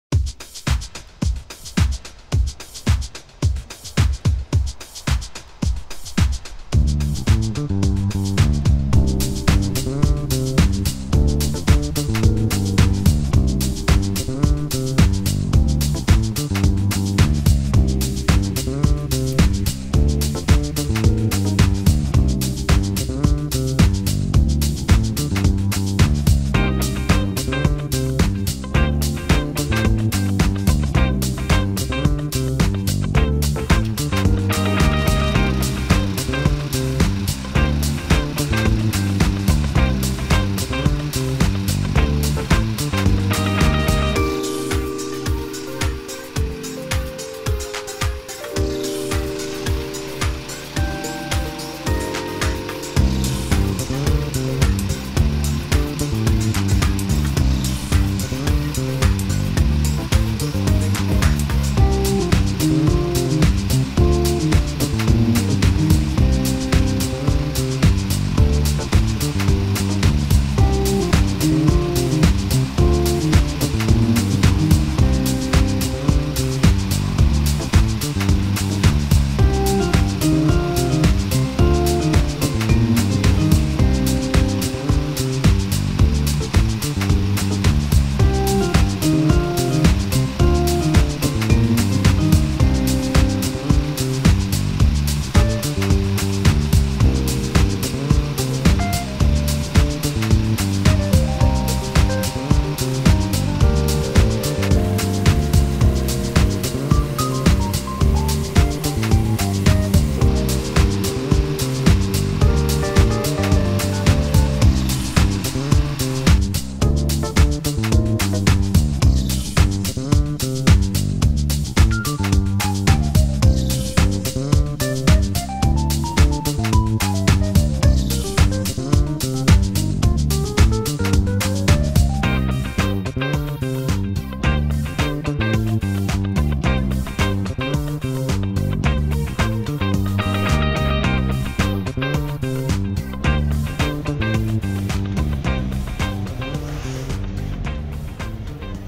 Mobiles Sandstrahlgebläse MTS 2.0
Einfach und schnell auf dem Friedhof Schriften nachstrahlen mit dem mobilen Sandstrahlgebläse MTS 2.0.
Zum Einsatz kam hier auch unser Benzinkompressor PETROREX.